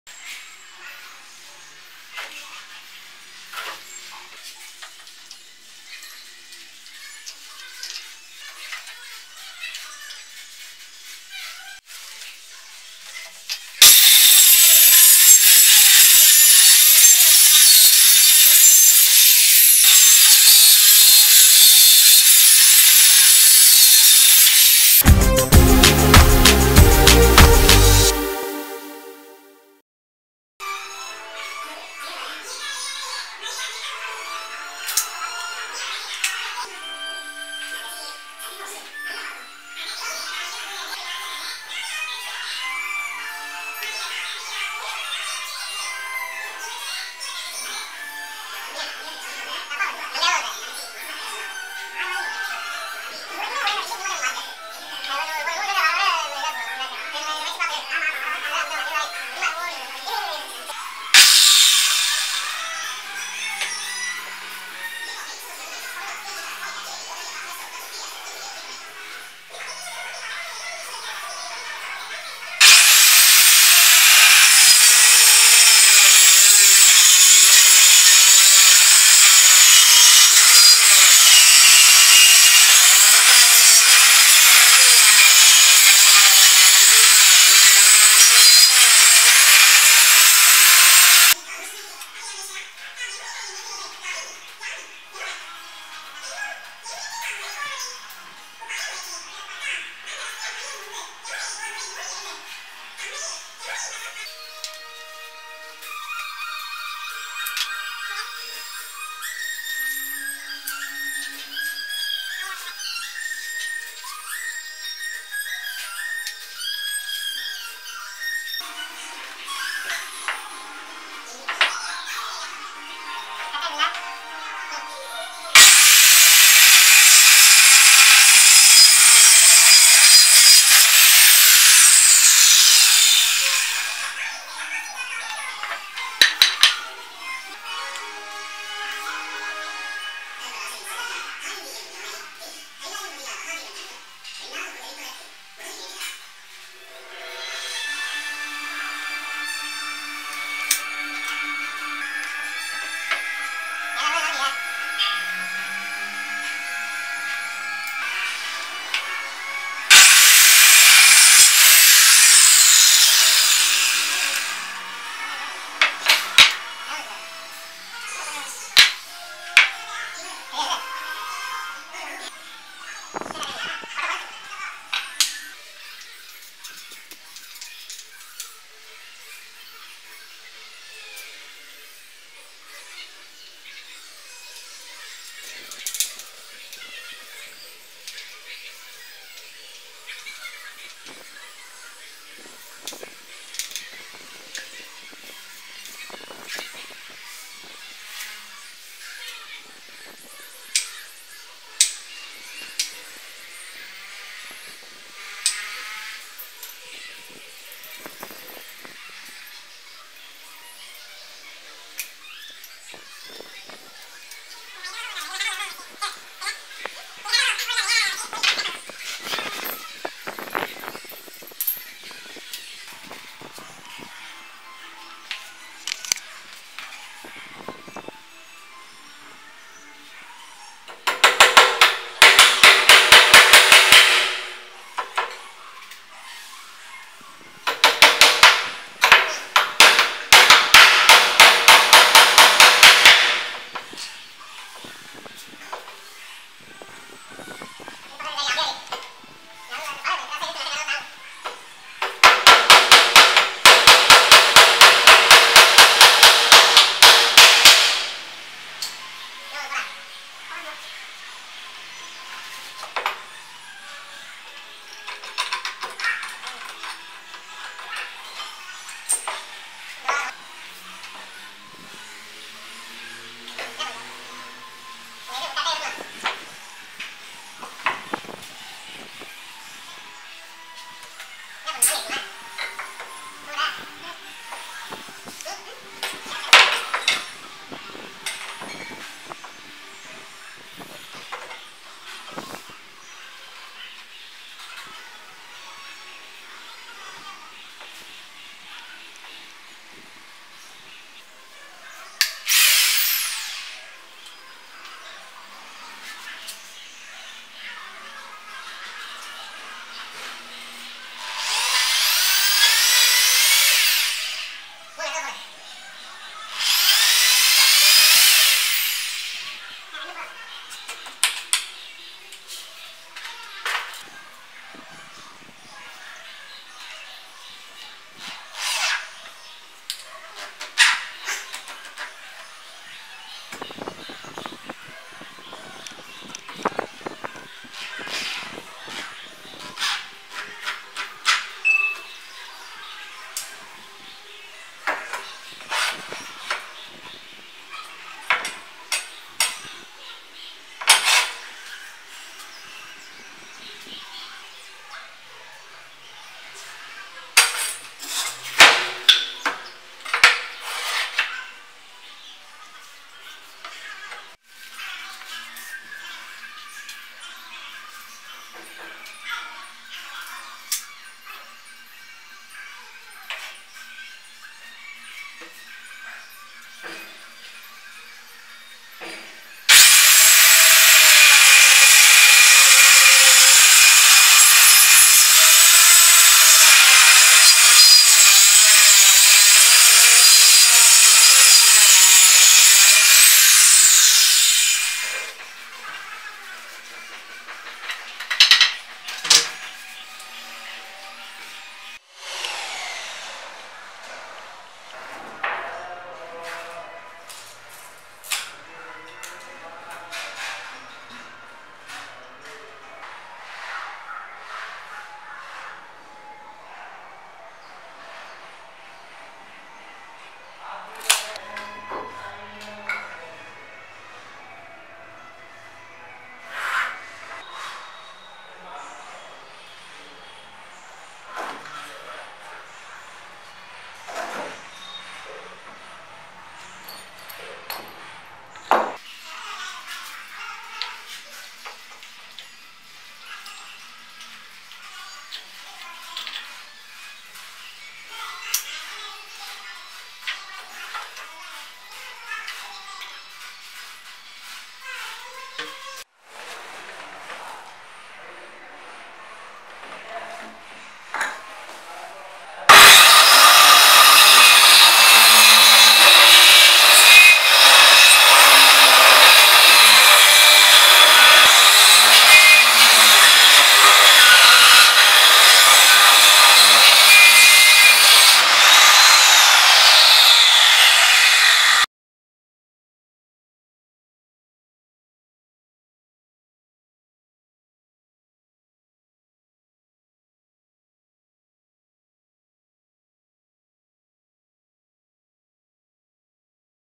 DIY Circular Saw Cutting Guide for Straight Cut | Simple Wood Working Tricks
Make Your Own Circular Saw Cutting Guide for Perfect Straight Cut using Simple Wood Working Tricks.